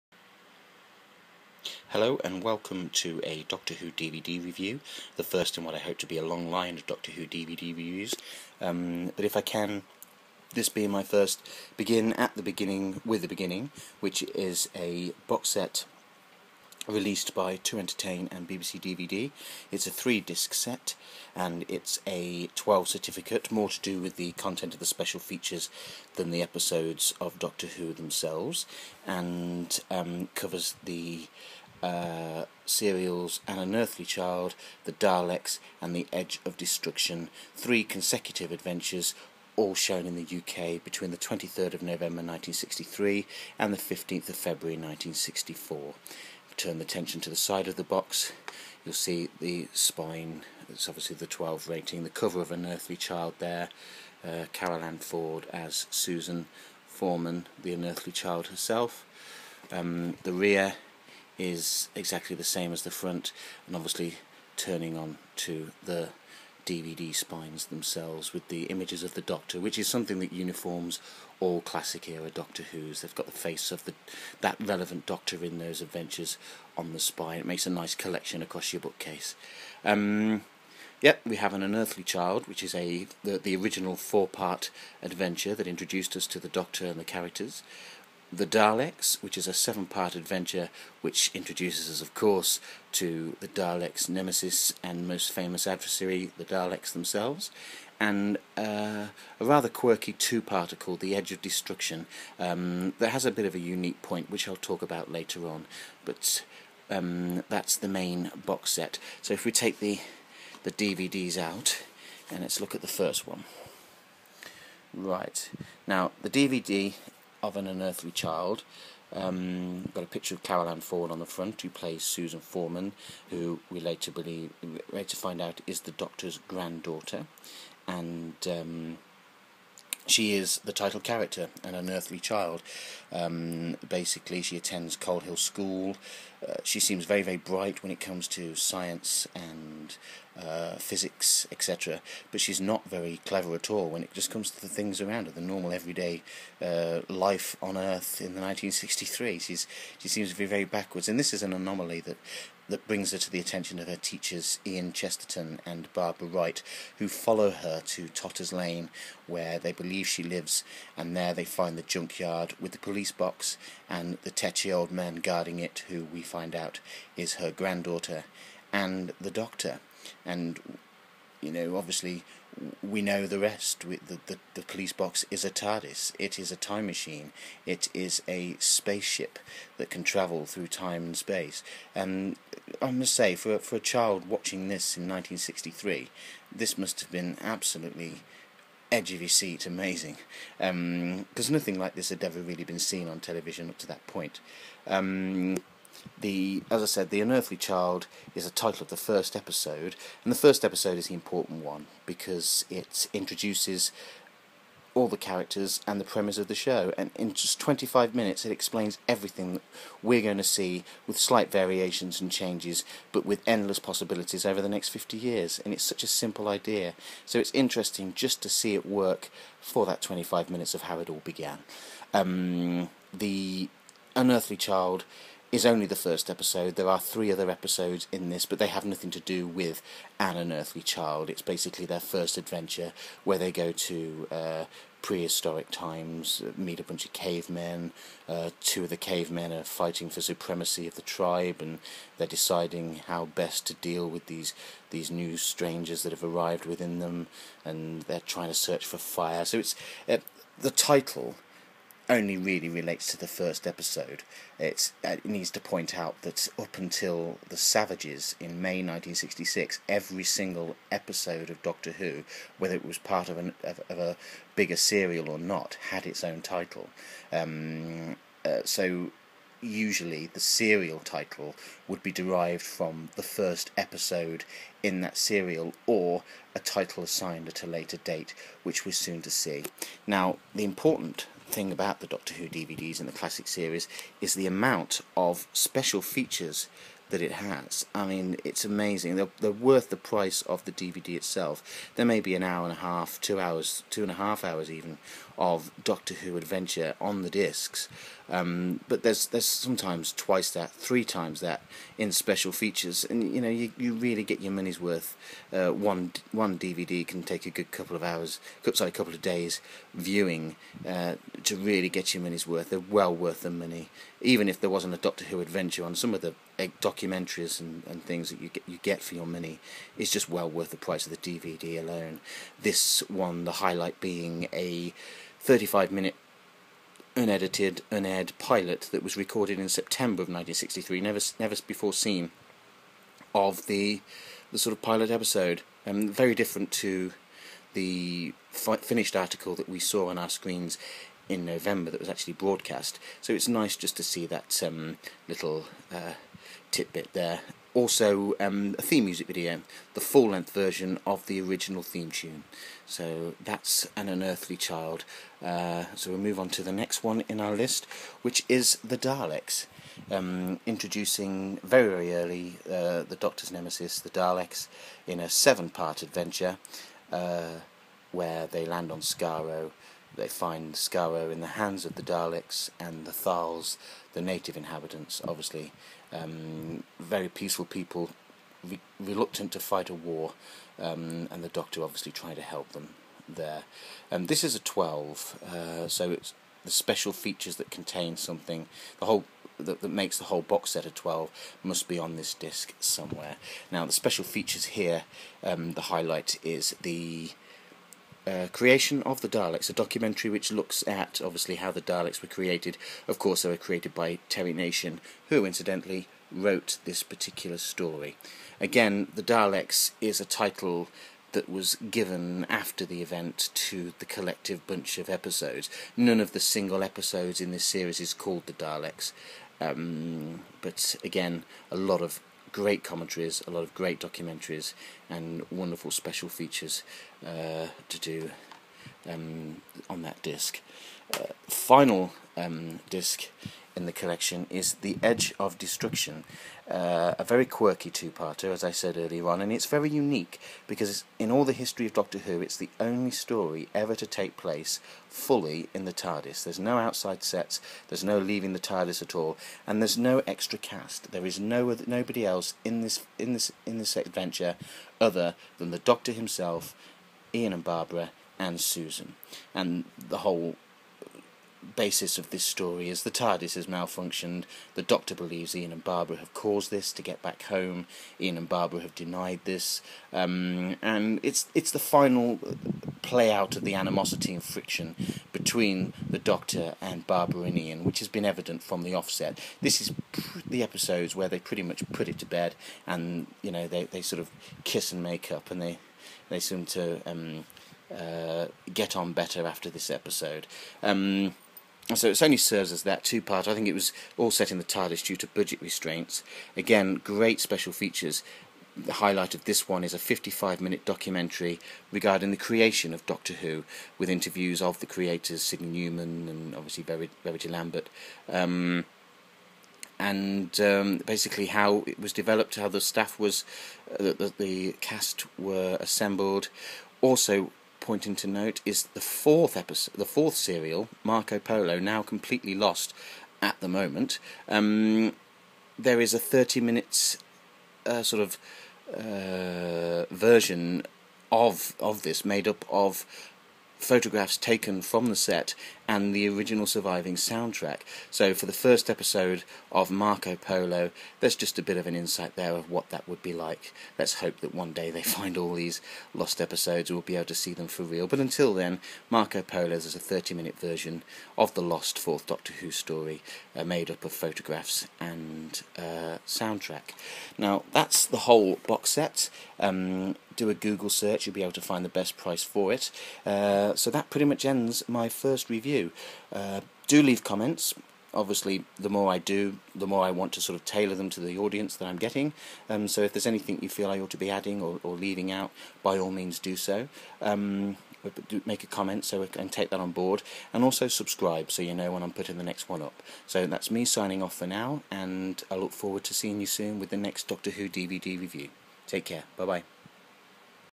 Doctor Who DVD Review 1
My Review of the Doctor Who Box set The Beginning featuring the first three Doctor Who serials An Unearthly Child, The Daleks and Edge of Destruction!